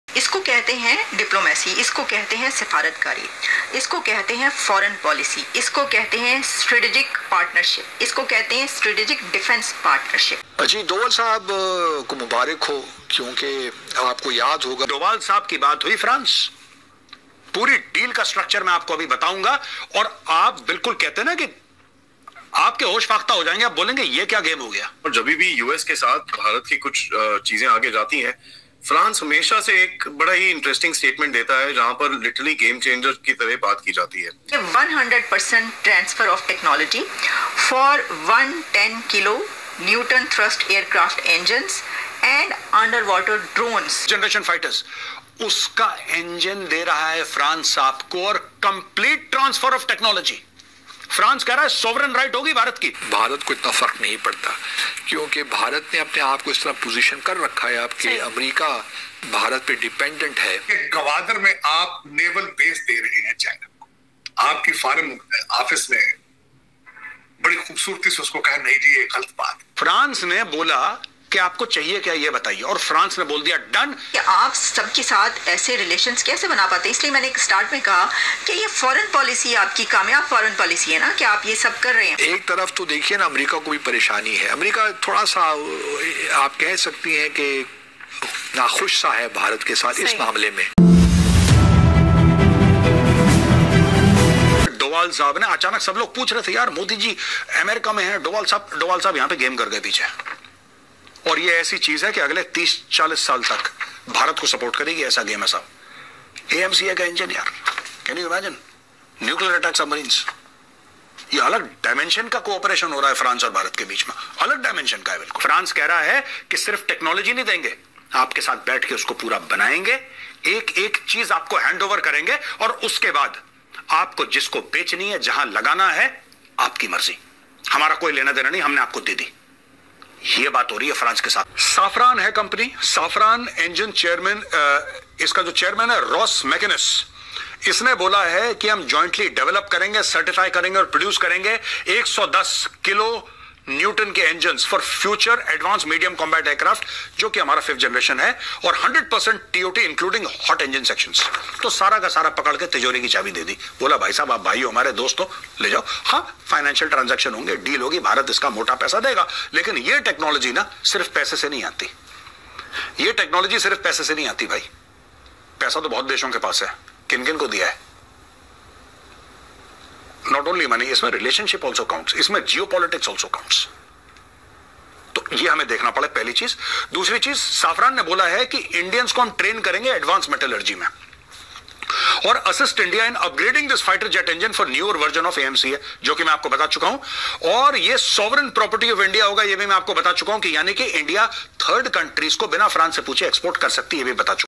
jiske liye US Bharat ka ghuma raha tha France ne Bharat ke liye khajana khol diya
pakmedia jealous
Hindustan special
now India speak